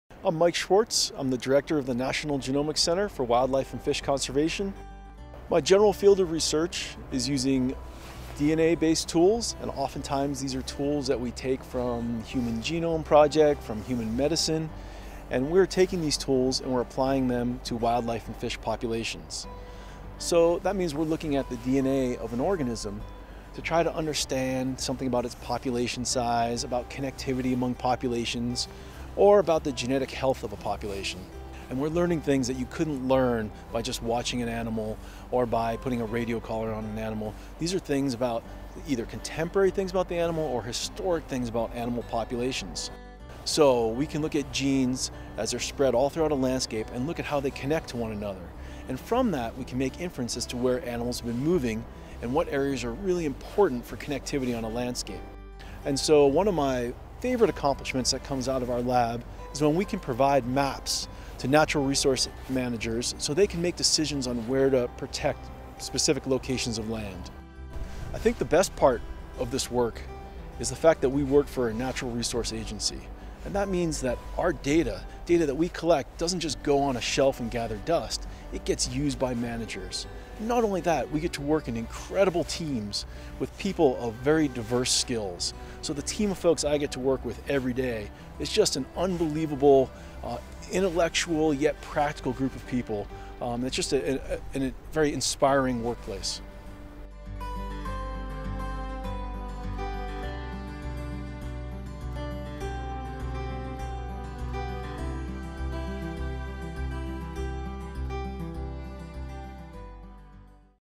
Meet Dr. Michael Schwartz, Director of the National Genomics
Dr. Michael Schwartz is a renowned research ecologist and was identified by Thomson-Reuters as one of the top influential scientific minds of 2015. Dr. Schwartz leads the new National Genomics Center for Wildlife and Fish Conservation in Missoula, MT. Learn what inspires his work in this new video.